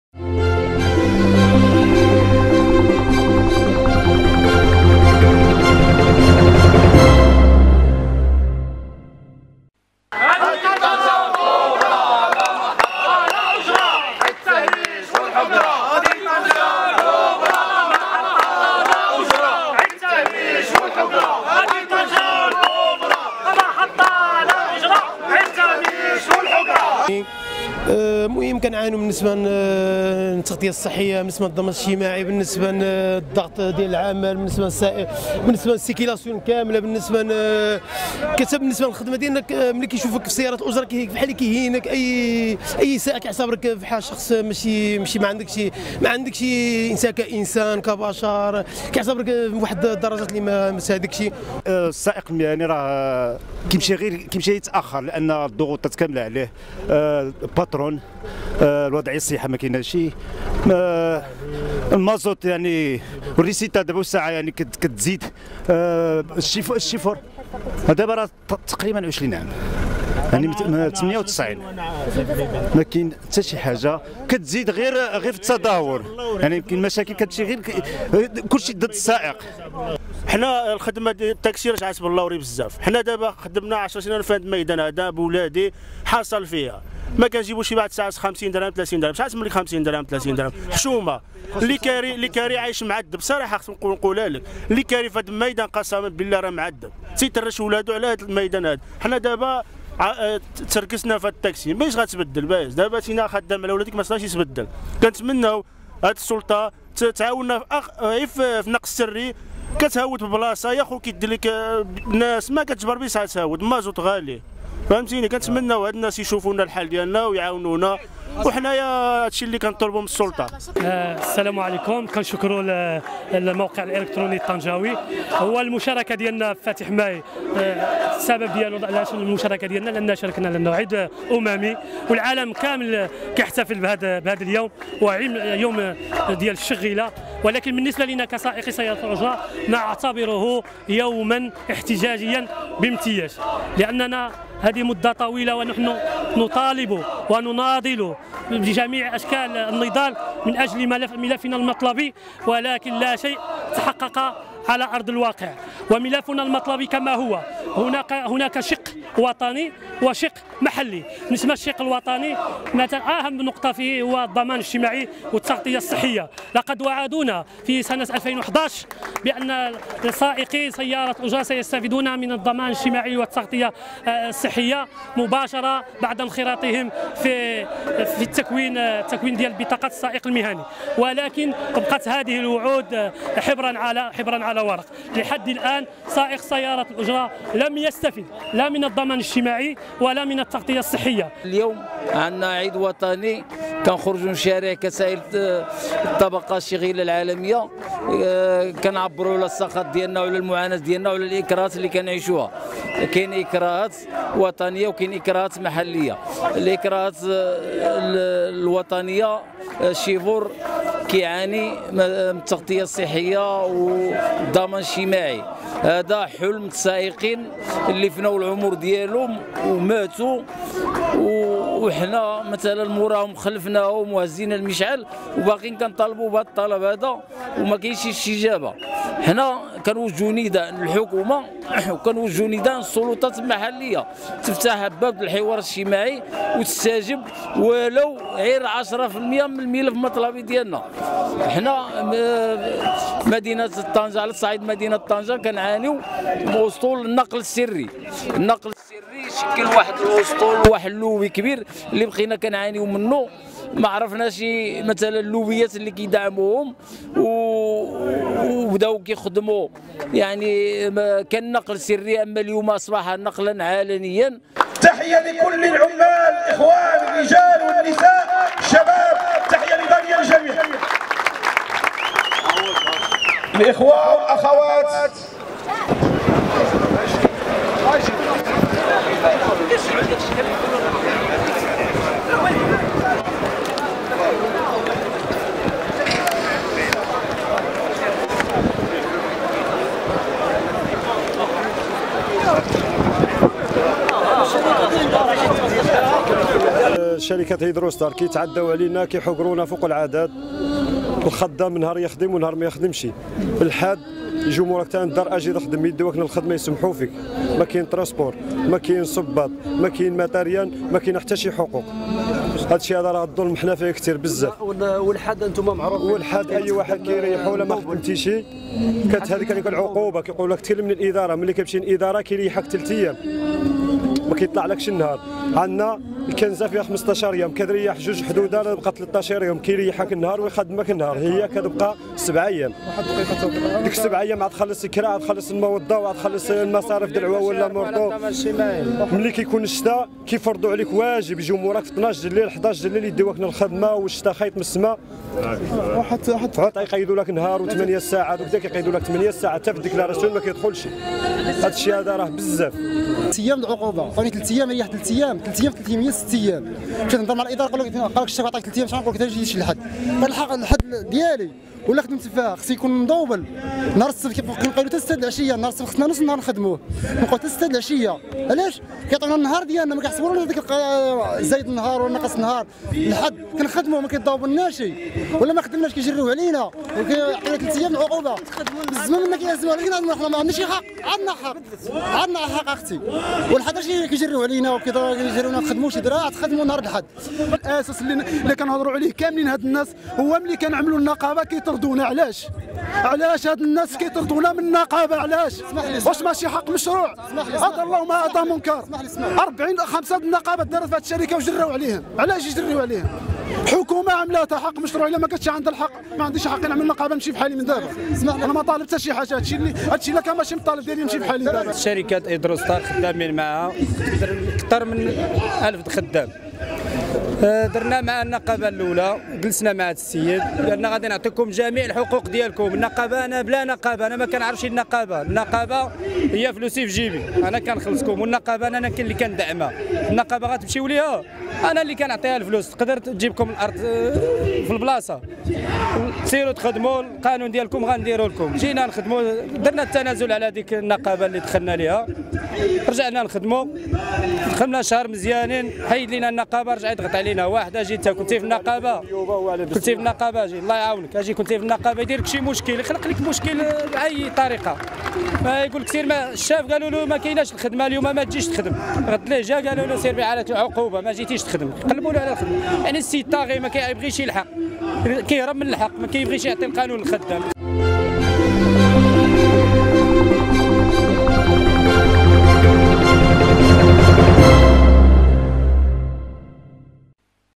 هكذا مرت اجواء احتفالات شغيلة طنجة بعيد العمال
أجواء باهتة في احتفالات فاتح ماي بطنجة
مرت احتفالات فاتح ماي بطنجة، وسط أجواء باهتة، بمدينة طنجة، حيث تراجعت تلك الاحتفالات الشعبية التي تكون فيها المشاركة مكثفة، من قبل الشغيلة، لأسباب عديدة، أجواء الملل التي شهدتها مدينة طنجة اليوم كسرتها أبواق سيارات الأجرة وعمال شركة النظافة و"أمانور".
 موقع "طنجاوي" خرج بدوره إلى الشارع ليرصد هذه الاحتفالات، وأعد الروبورتاج التالي.